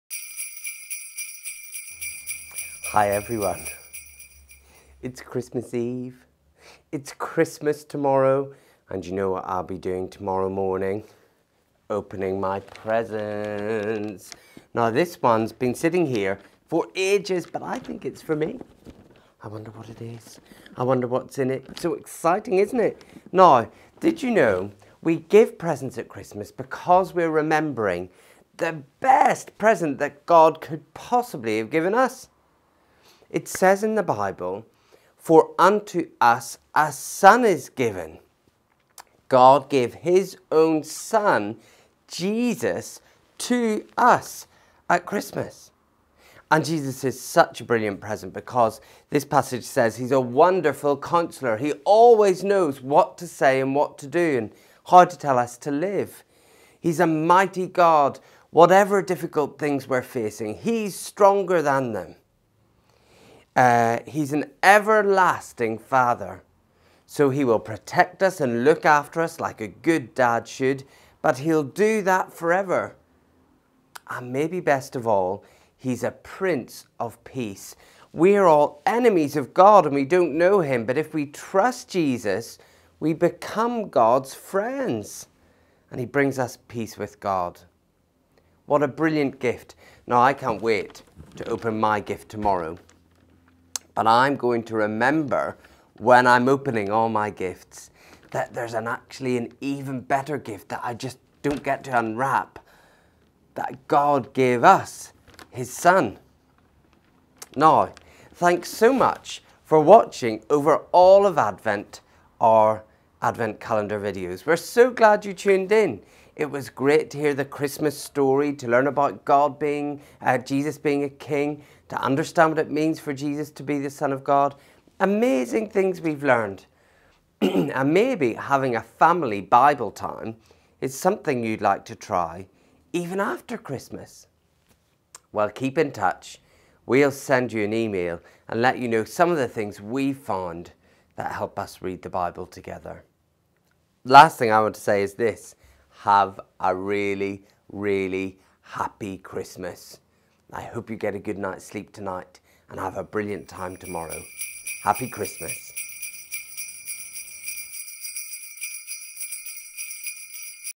The Great Advent Calendar | Day 24
𝗔𝗻 𝗲𝘅𝗰𝗶𝘁𝗶𝗻𝗴 𝘄𝗮𝘆 𝗳𝗼𝗿 𝗳𝗮𝗺𝗶𝗹𝗶𝗲𝘀 𝘁𝗼 𝗲𝘅𝗽𝗹𝗼𝗿𝗲 𝘁𝗵𝗲 𝘀𝗽𝗶𝗿𝗶𝘁𝘂𝗮𝗹 𝘀𝗶𝗱𝗲 𝗼𝗳 𝗖𝗵𝗿𝗶𝘀𝘁𝗺𝗮𝘀 𝗲𝘃𝗲𝗿𝘆 𝗱𝗮𝘆 𝘁𝗵𝗶𝘀 𝗗𝗲𝗰𝗲𝗺𝗯𝗲𝗿.

Welcome to day 24 of the The Great Advent Calendar. We're so excited that you've joined us.

This is one of many events we have going on this Christmas, for more check out